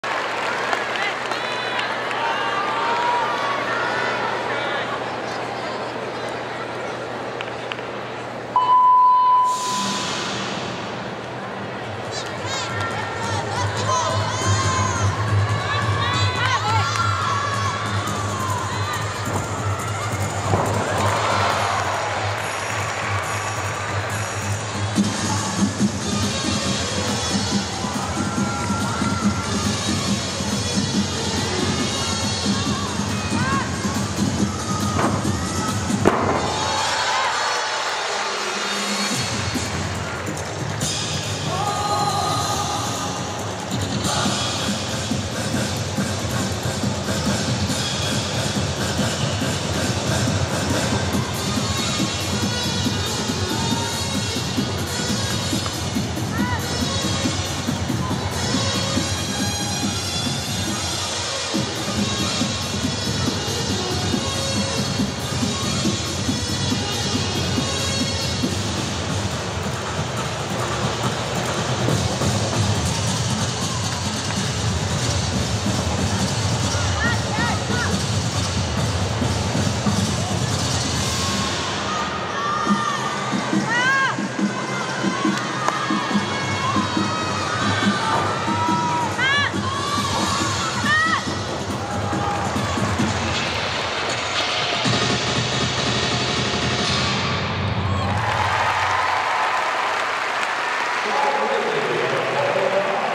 Skye Blakely - Floor Exercise - 2023 Xfinity U.S. Championships - Senior Women Day 1
Score: 12.900 (D: 5.6, E: 7.400, ND: -0.1)
August 25, 2023 - SAP Center - San Jose, Calif.